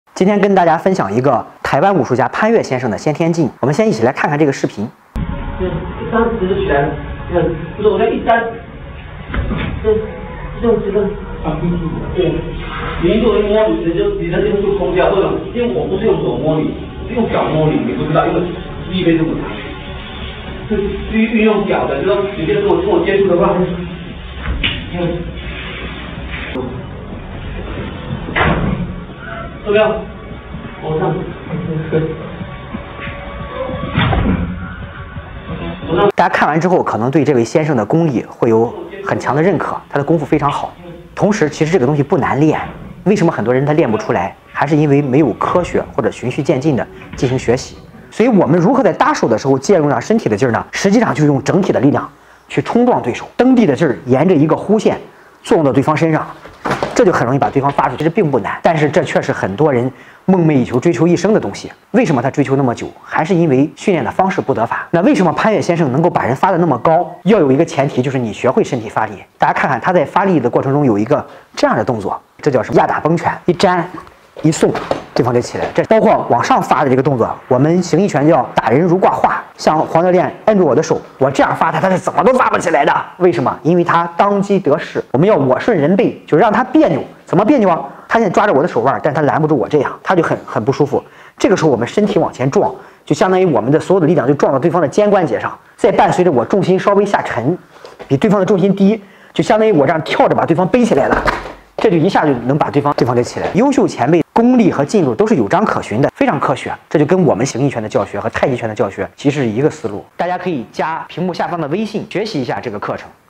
八卦掌潘岳前辈的先天劲，你没见过的内家拳劲
啥是内家拳的先天劲？庞超老师带你看看八卦掌潘岳前辈的发力 ，你没见过的内家拳劲